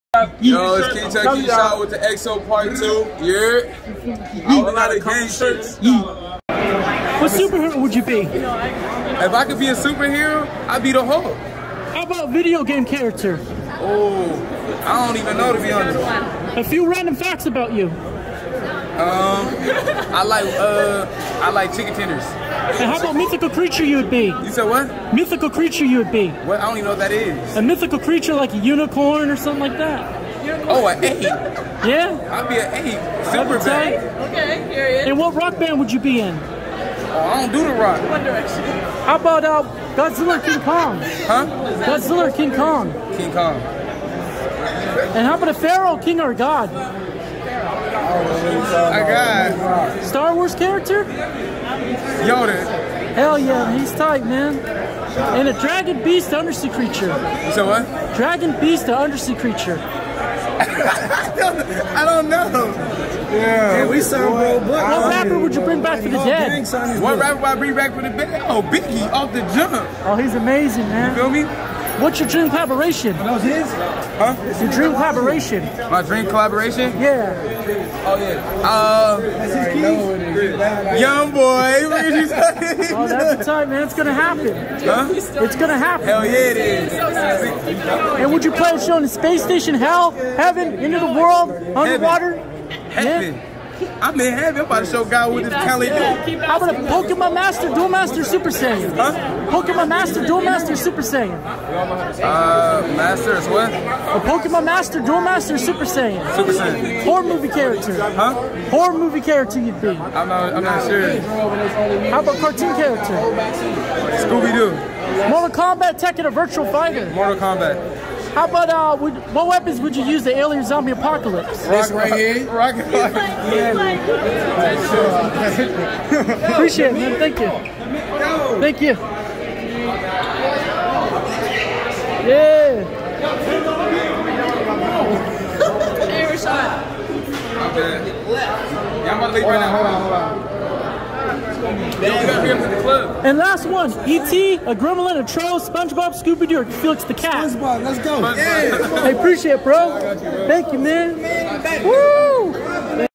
TheXLTE part 2 interviews Kingtut Kingshad June 5th 2021
Lil Tjay Destined 2 Win Tour @ The marquee Theater In Tempe Az